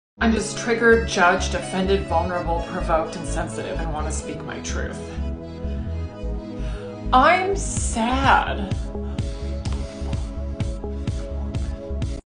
Which one are you? - CARMEN LYNCH
Back in the day we only had one word. Then we had to break it down in therapy in our thirties. Times have changed 😂😂

More on Instagram and TikTok @carmencomedian and podcast The Human CentiPOD on all platforms.

SEND TO A GEN X OR A GEN Z.